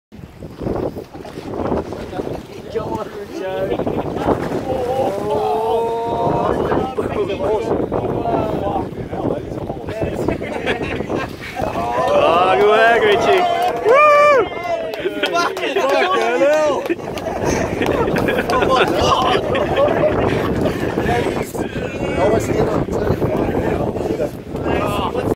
Pro Red Fishing Charters Melbourne
A Big Melbourne Snapper aboard Pro Red Fishing Charters